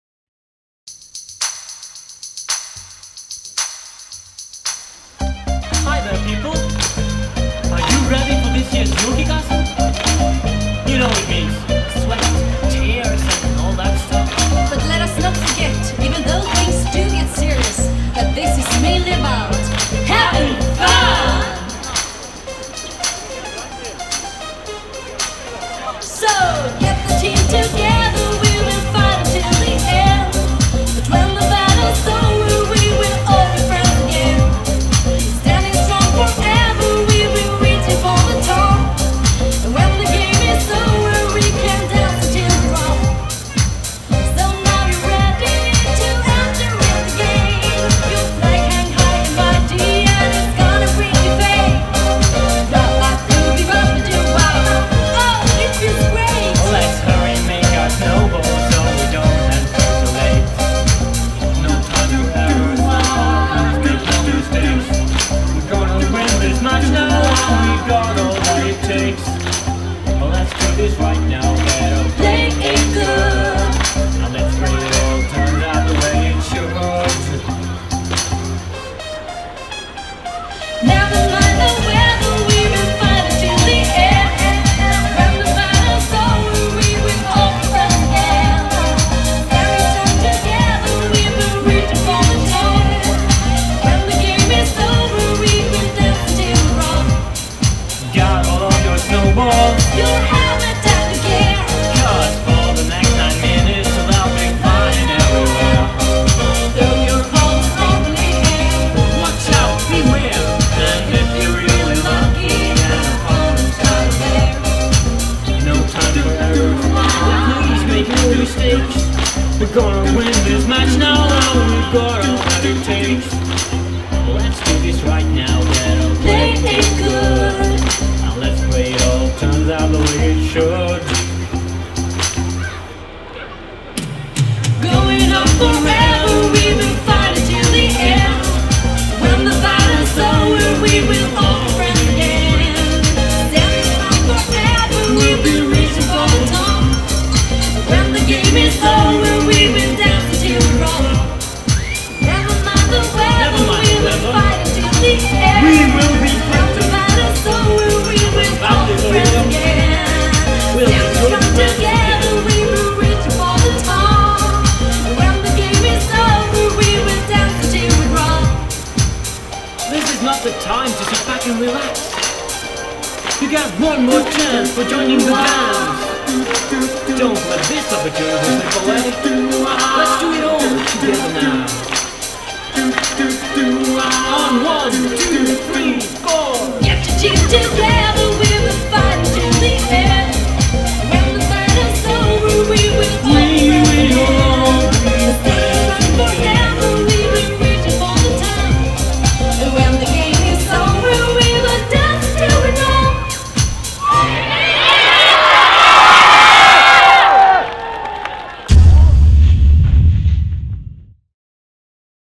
VG-lista til Vardø - Yukidans
I snøvær og med en liten dose vardøvind stilte godt over 15% av byens befolkning opp på minuttet for å få